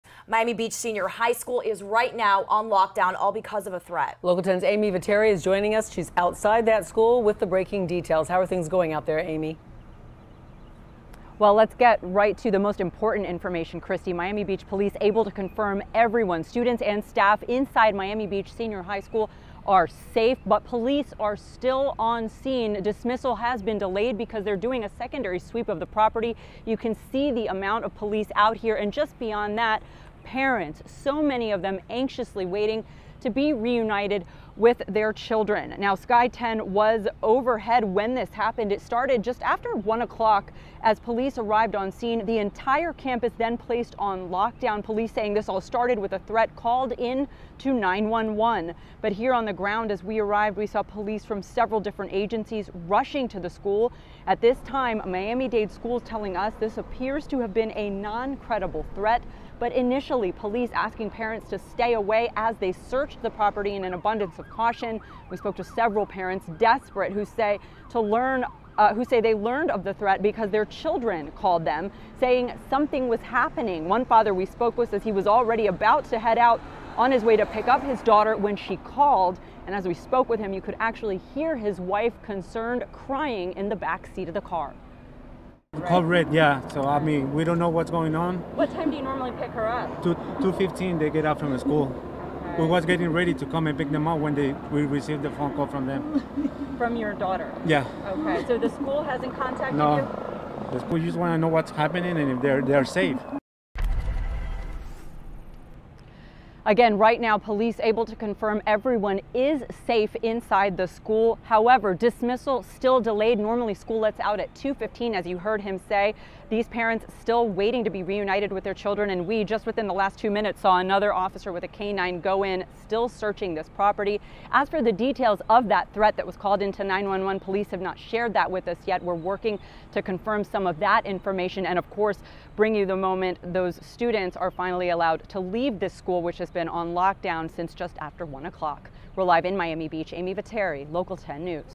Officers lockdown Miami Beach Senior High School
Officers locked down Miami Beach Senior High School on Wednesday afternoon for about two hours to conduct security sweeps after someone reported a threat.